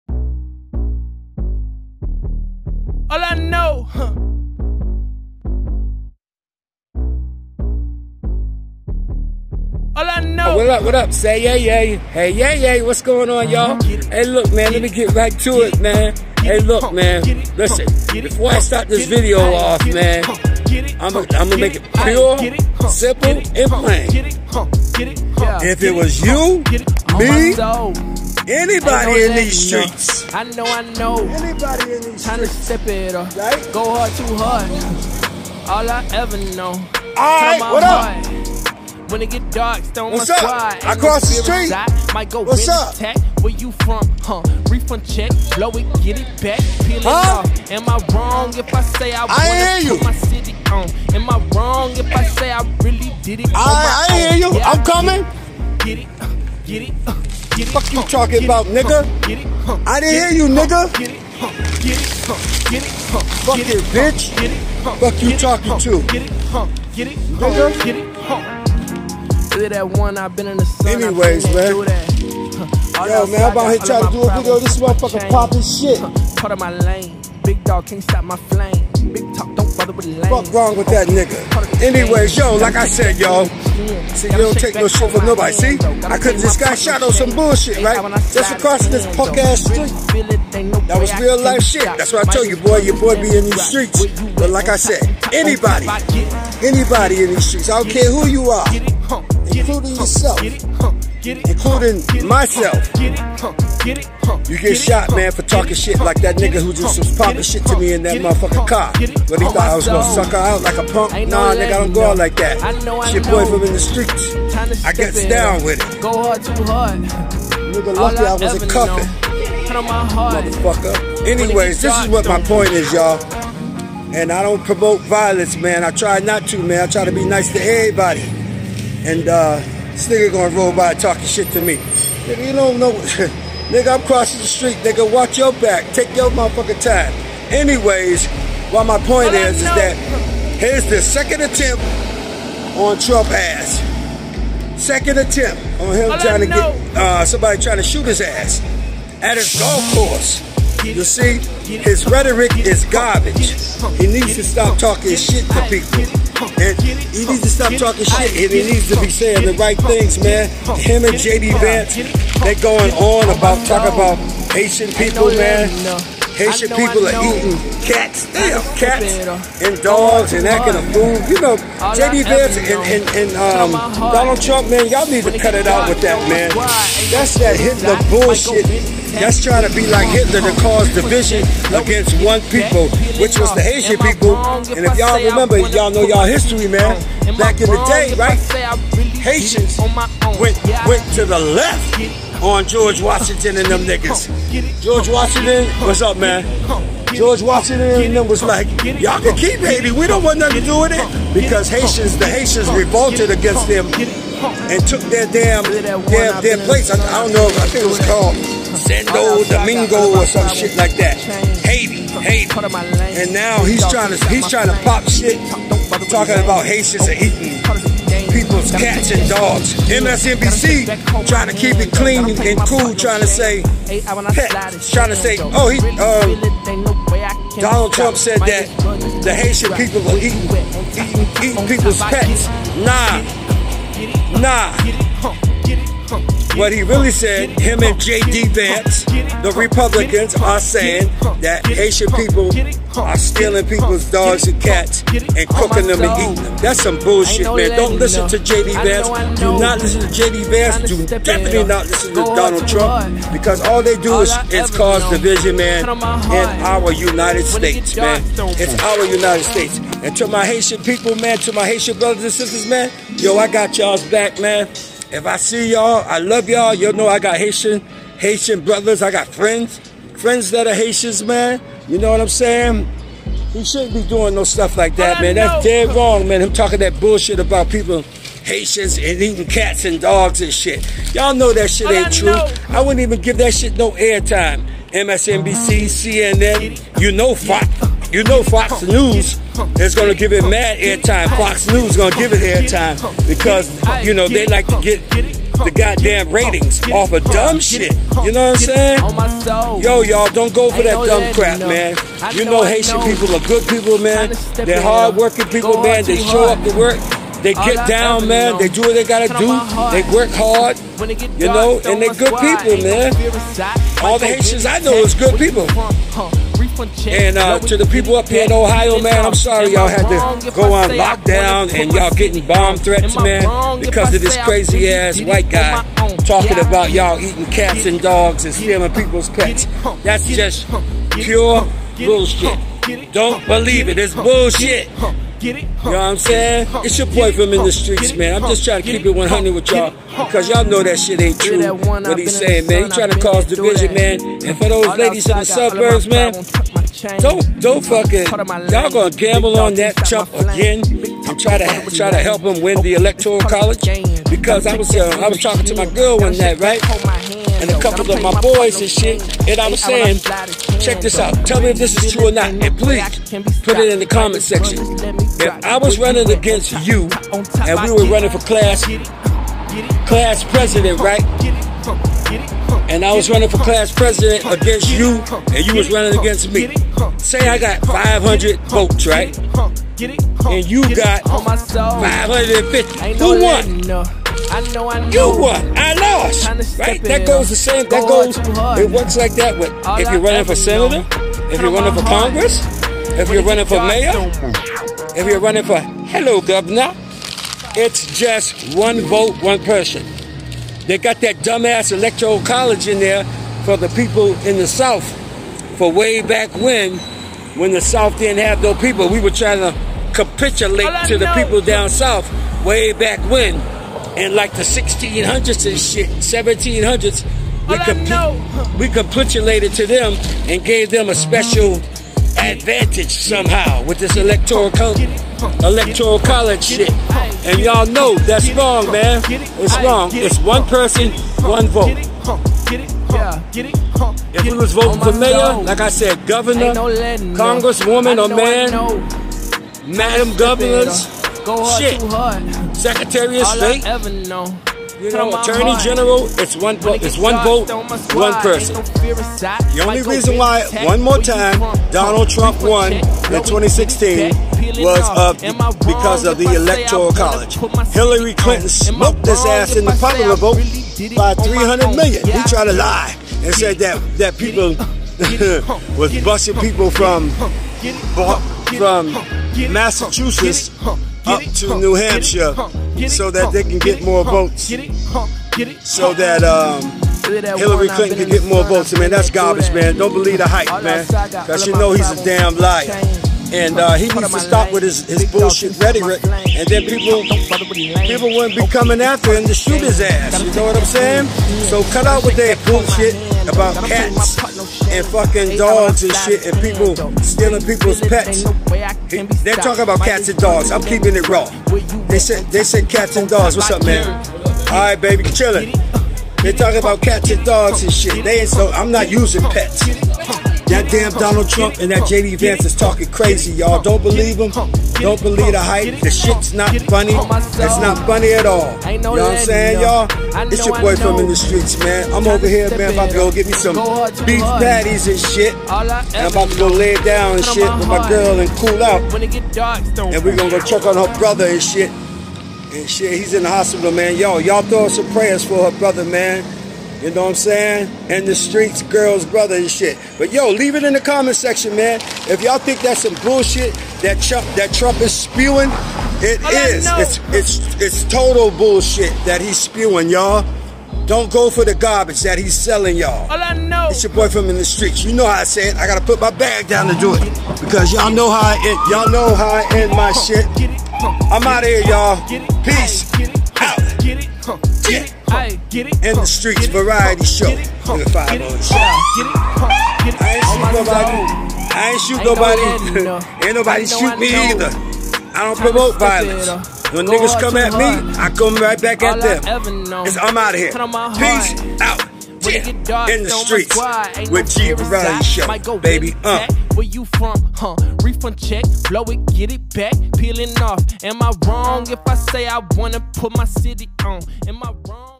REPUBLICANS STOP PUSHING HATE SPEECH LEAVE HAITIANS ALONE!
TRUMP, JD VANCE, & REPUBLICANS NEED TO STOP LYING ON HAITIANS.